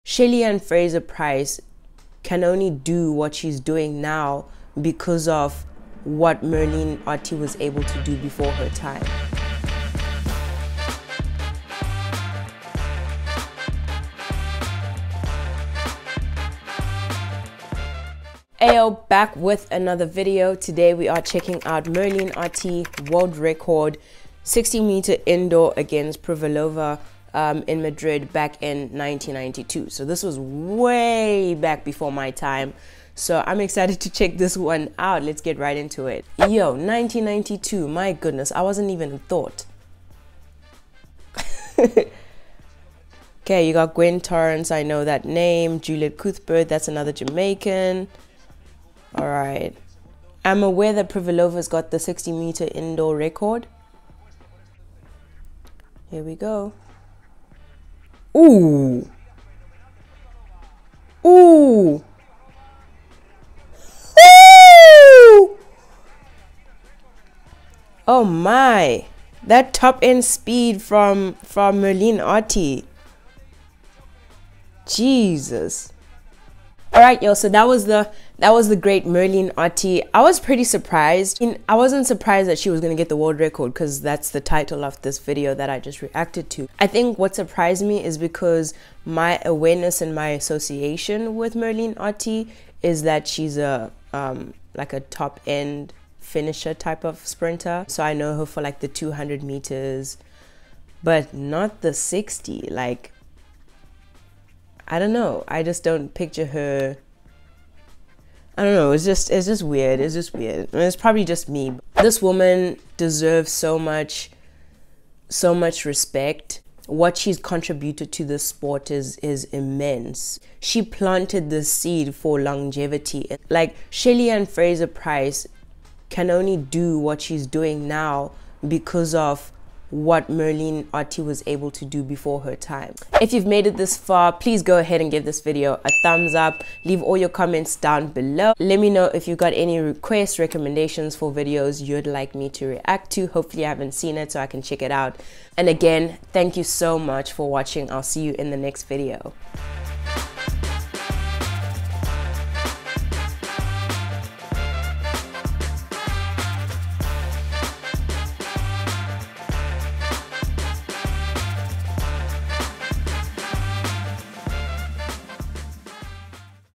Merlene Ottey Breaks 60m WORLD RECORD | Madrid 1992 | REACTION
Hey guys! In this video I react to Merlene Ottey's 60m Indoor World Record performance in Mardrid 1992.

TIMESTAMPS:
00:00 Preview
00:09 Intro
00:19 Welcome
00:38 Reaction
01:17 Post-Reaction Thoughts
02:27 Outro